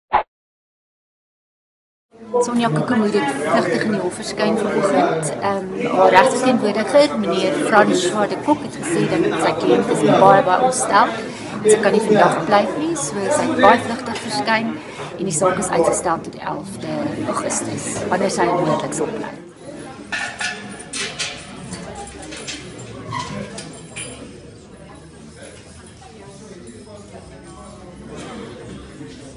Dogter dood gewurg: Ma te ontsteld om te pleit
Soniya Koekemoer het Donderdagoggend vlugtig in die hof verskyn.Haar regsverteenwoordigers sê dat sy te ontsteld is om nou te pleit.Haar ouers was daar om haar te ondersteun. Die saak is tot 11 Augustus uitgestel wanneer sy moontlik sal pleit.